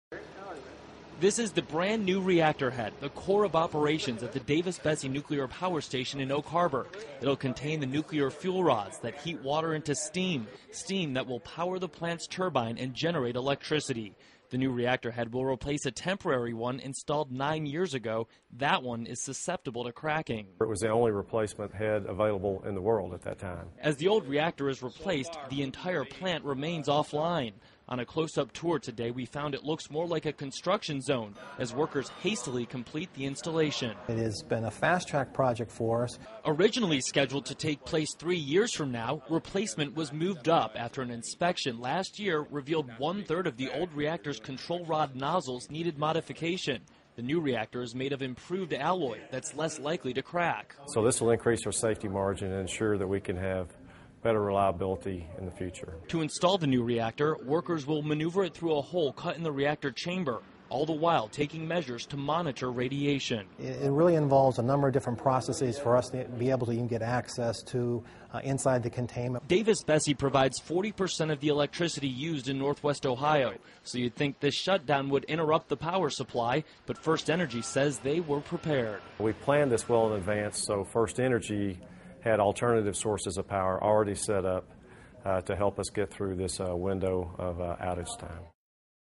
OHIO REPLACING a Bad NUCLEAR REACTOR HEAD
Concerns have gone on for years regarding the reactors of SE Michigan and NW Ohio.  Recent discoveries of failing parts have prompted the commission of a new reactor head for davis-besse nuclear plant in Ohio.  They have relied on a temporary head, of low quality, to continue operations at an "exceptable risk" to the plant and public.